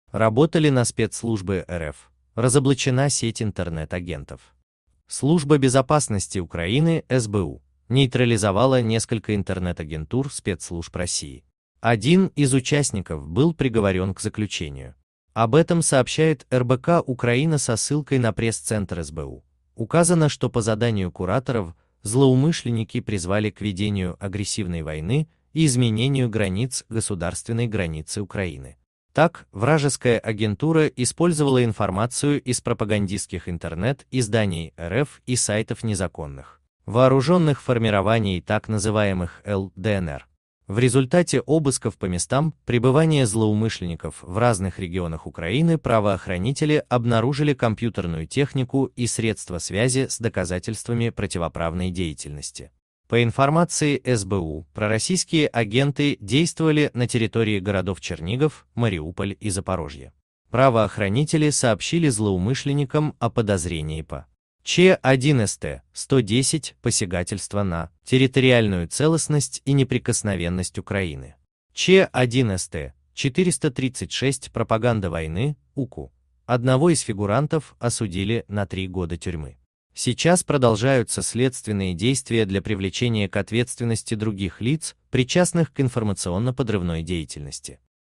Работали на спецслужбы РФ: разоблачена сеть интернет-агентов.
СлужбабезопасностиУкраины Интернет Служба безопасности Украины (СБУ) нейтрализовала несколько интернет-агентур спецслужб России. Один из участников был приговорен к заключению. Об этом сообщает РБК-Украина со ссылкой на пресс-центр СБУ. Указано, что по заданию "кураторов" злоумышленники призвали к ведению агрессивной войны и изменению границ государственной границы Украины. Так, вражеская агентура использовала информацию из пропагандистских интернет-изданий РФ и сайтов незаконных вооруженных формирований так называемых "Л/ДНР". В ре